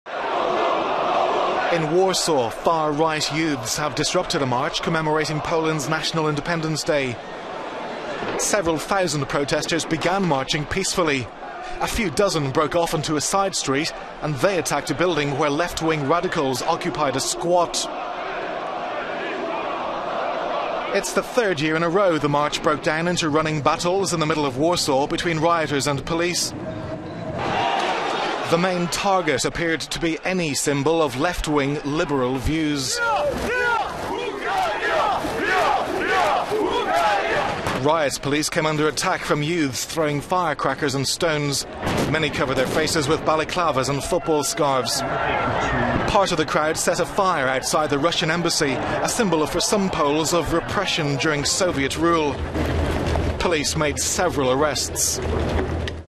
Poland Clashes erupt between far right protesters and left wing squatters
In Warsaw, Polish Independence Day celebrations turned violent for the third year in a row following an attack on a left-wing squat by a group of far-right youths.

It's the third year in a row the march broke down into running battles in the middle of Warsaw between rioters throwing rocks and petrol bombs and police using rubber bullets, truncheons and pepper spray.

Part of the crowd set a fire outside the Russian embassy -- a symbol for some Poles of repression during soviet rule.